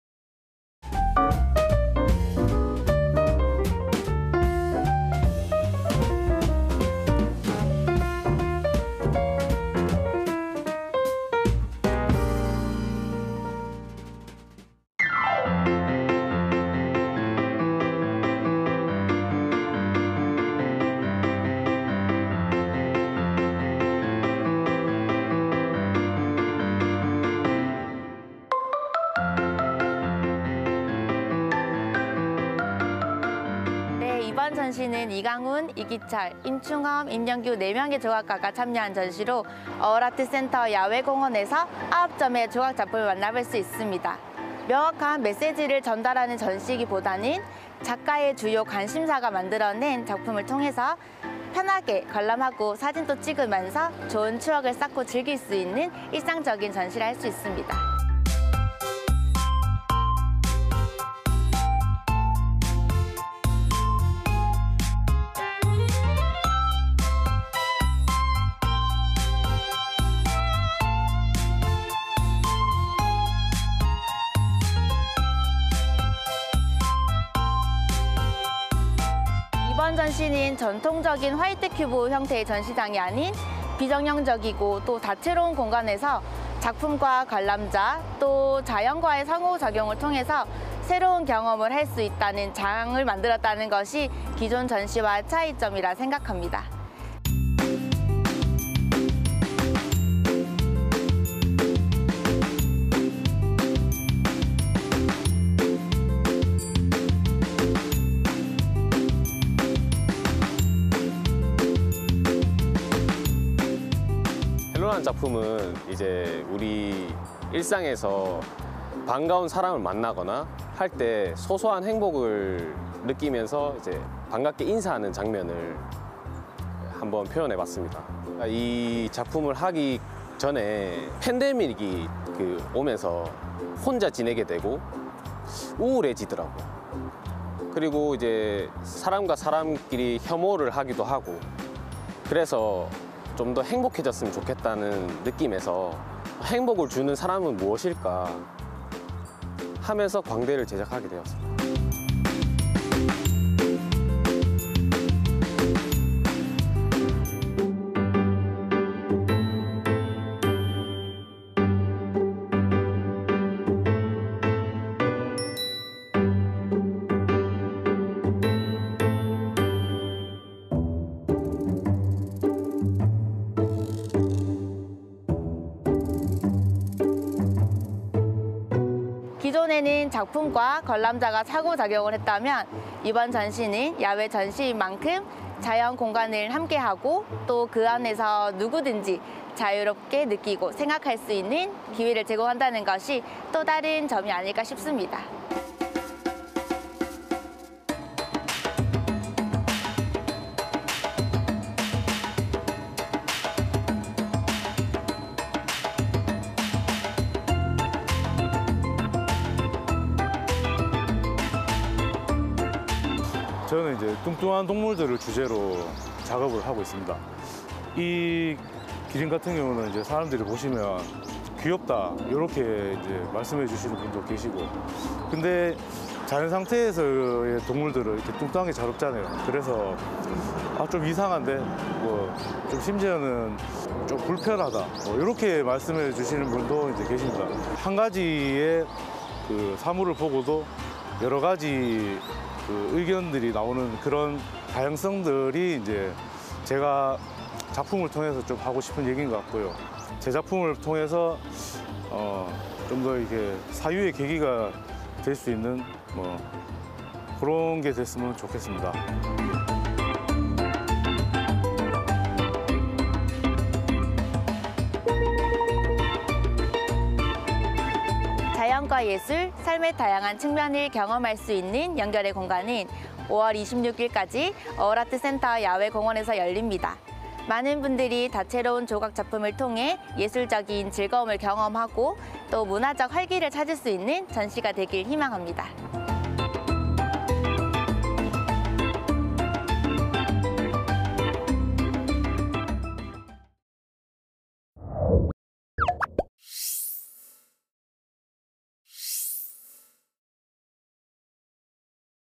[문예현장] 비정형큐브 야외조각전 ‘연결의 공간’ / KBS 2024.04.11.
대구 북구어울아트센터는 다음 달(5월) 26일까지 야외공원에서 비정형 야외조각전 '연결의 공간'을 진행합니다.

이번 전시는 전통적인 화이트 큐브 형태의 전시장이 아닌 비정형적이고 다채로운 야외 공간에서 자연을 배경으로 마련됐습니다.

관람객들은 24시간 자유롭게 조각 작품을 감상할 수 있고, 작가 역시 예술과 자연이 합쳐진 공간에서 작품이 시너지를 발휘하는 새로운 경험을 할 수 있을 것으로 기대됩니다.